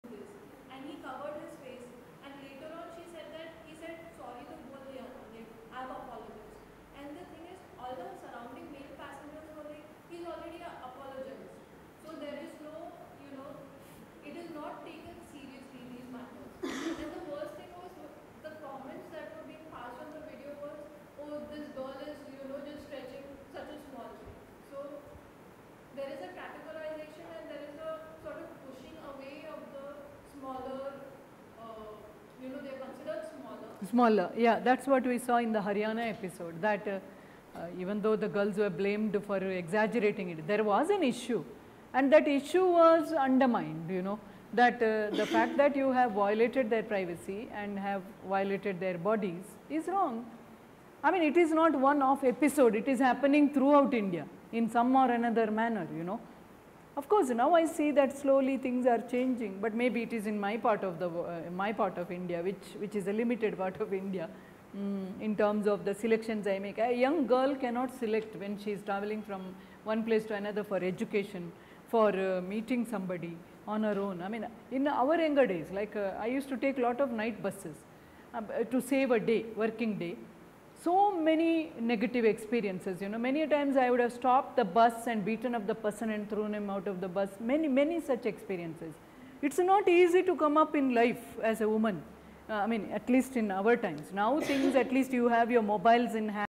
Dr Shashikala Gurpur - SLS Pune
Dr. Shashikala Gurpur – On Human Rights of Women for LLM Students. (3)